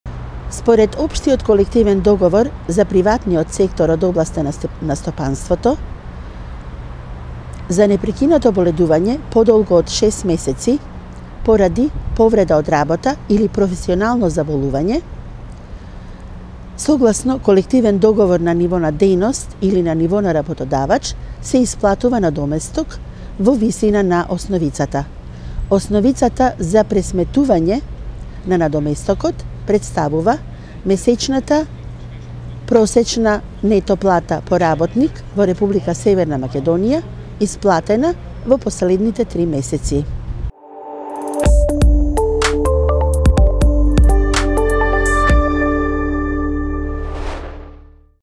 Дали вработен со повеќе операции има право на материјална помош?
Одговара Весна Кочовска, експерт за трудово право.